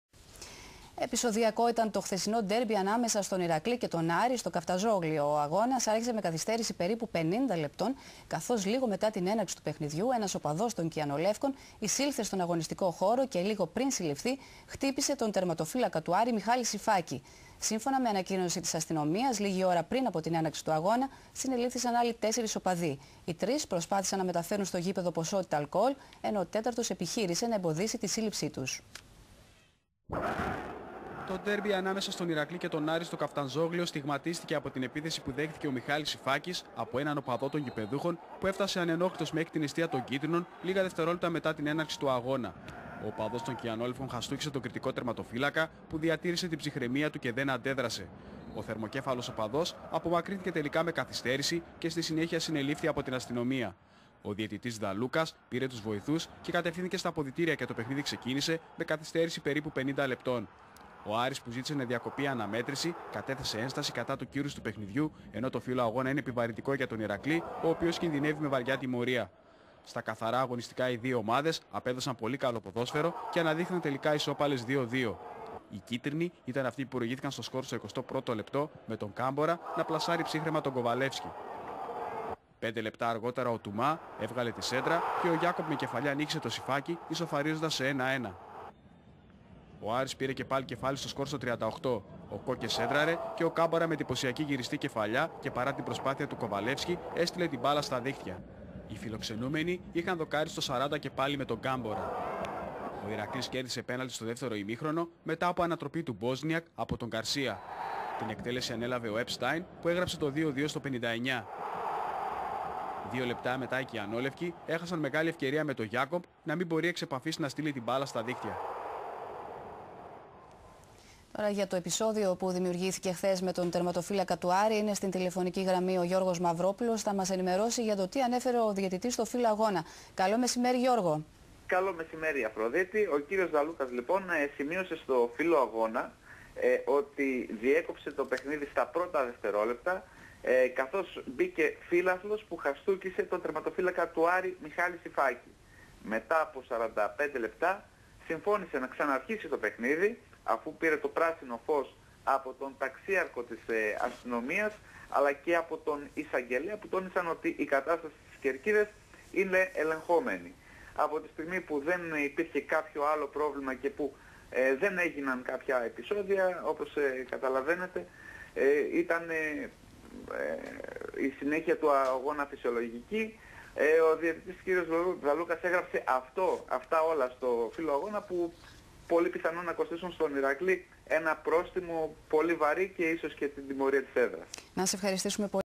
Iraklis-ARIS 2-2 (2009/10)
Iraklis-ARIS 2-2

2 agwnistiki 2009-10

0-1 Campora 20'
1-1 Iacob 25'
1-2 Campora 37'
2-2 Epstein 59'